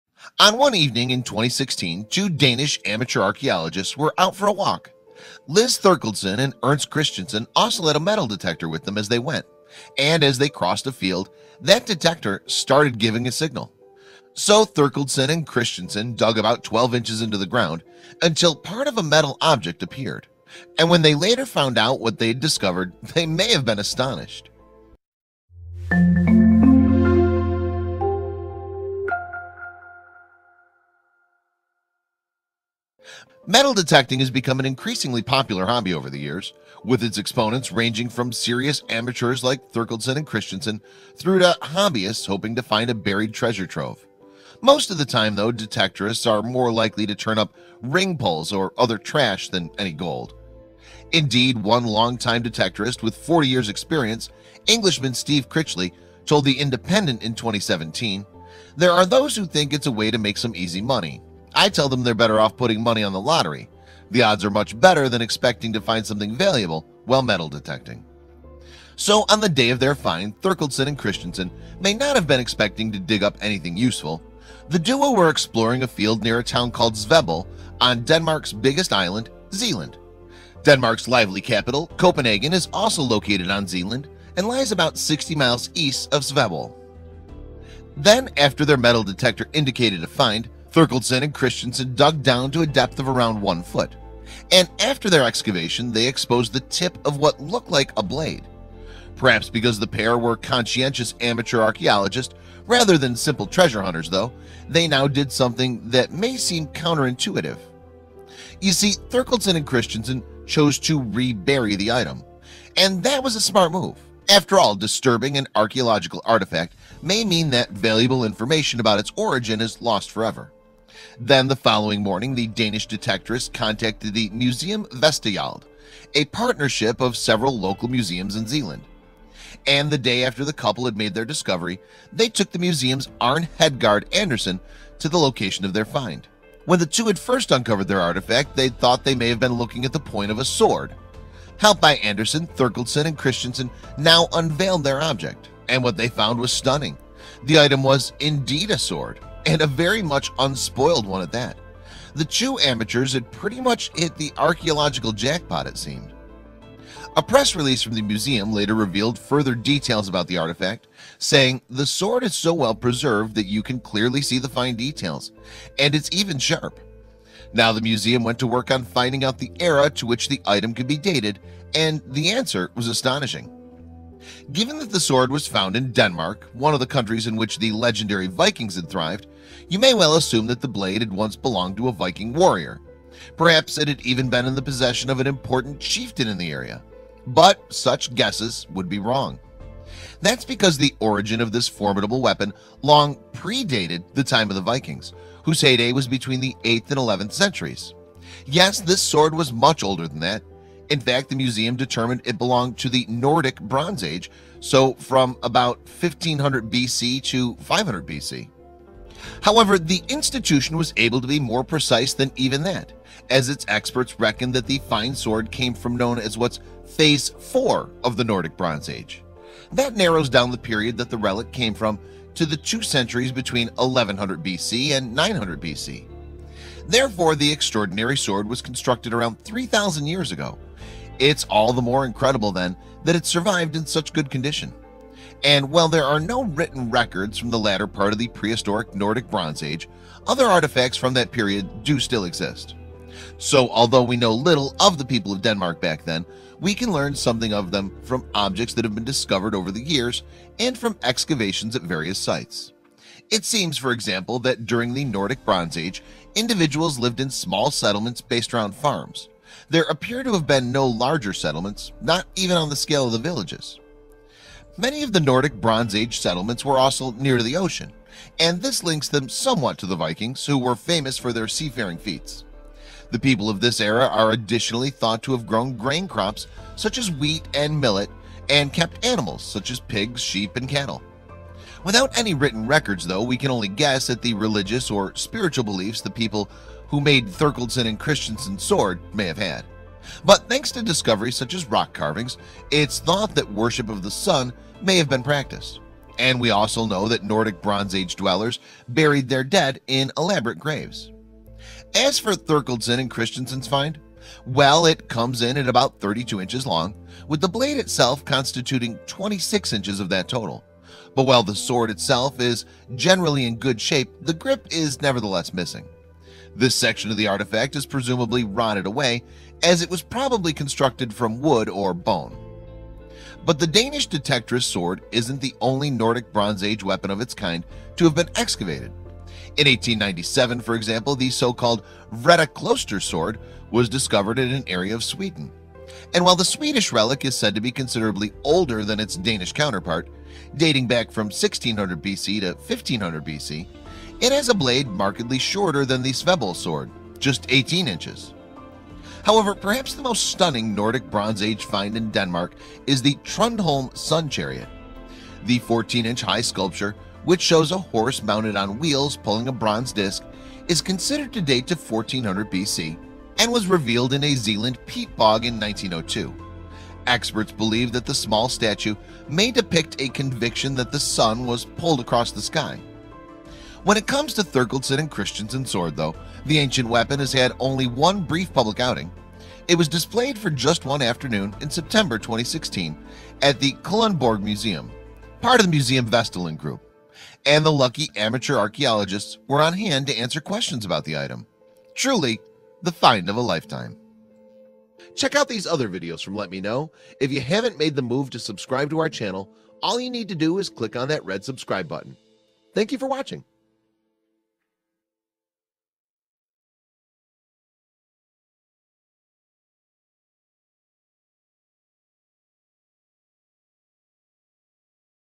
A Danish Couple Out Using Their Metal Detectors Discovered An Immaculate 3,000 Year Old Weapon
On one evening in 2016, two Danish amateur archaeologists were out on a walk. Lis Therkildsen and Ernst Christiansen also had a metal detector with them as they went, and as they crossed a field, that detector started to give a signal. So, Therkildsen and Christiansen dug about 12 inches into the ground until part…

►Image credits:
Image: Jeff Fusco/Getty Images
Image: Stefan Sauer/AFP/Getty Images
Image: Peterborougharchaeology.org
Image: Søren Kiehn, Museum Vestsjælland
Image: Feline
Image: Henning Bagger/AFP/Getty Images